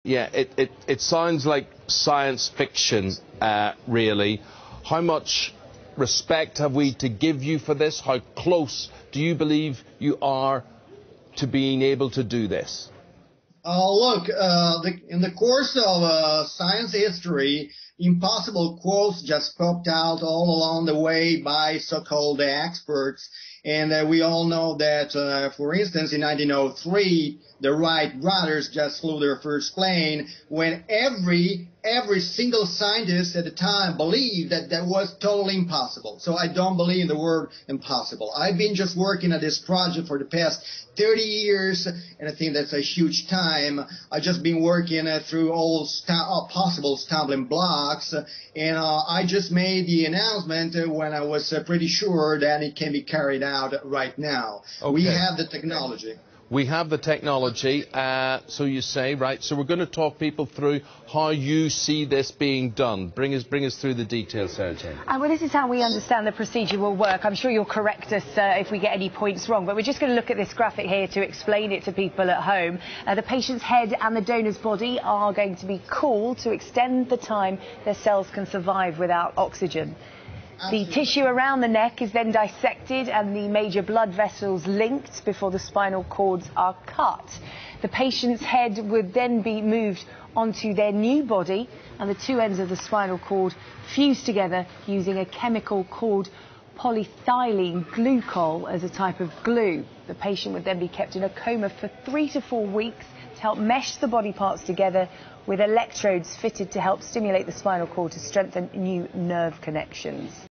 Head Transplant: Science Fiction Or Reality?
Neurosurgeon Dr Sergio Canavero says "we have the technology" to transplant heads in the near future.